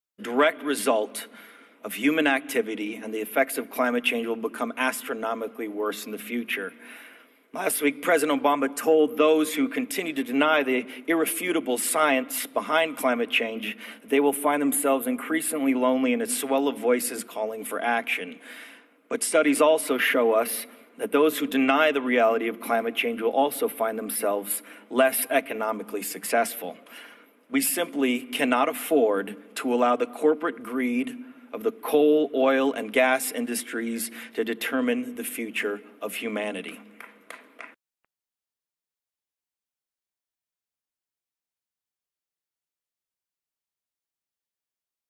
Leonardo DiCaprio, l'hypocrite environnemental ultra-riche qui nous fait la leçon depuis son yacht
Pour les élites hypocrites comme DiCaprio, vous êtes un monstre si vous conduisez un Ford F-150. Mais lui, il passe son temps dans les yachts et les jets privés!